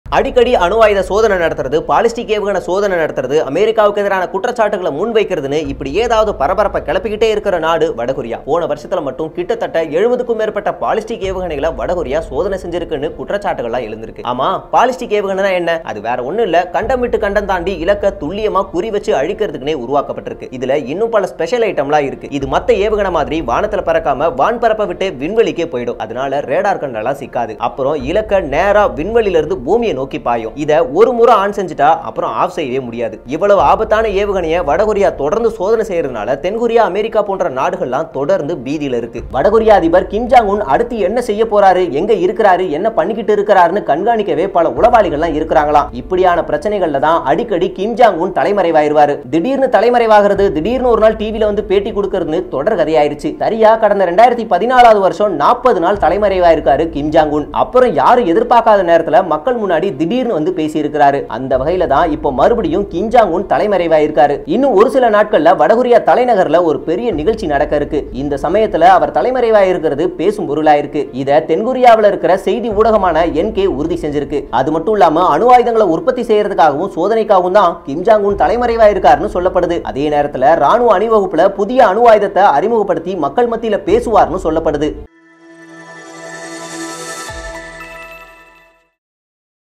மீண்டும் தலைமறைவான அதிபர், அதிரும் அமெரிக்க அடுத்து என்ன நடக்கும்? | Kim Jong-un
70க்கும் மேற்பட்ட ஏவுகணை சோதனை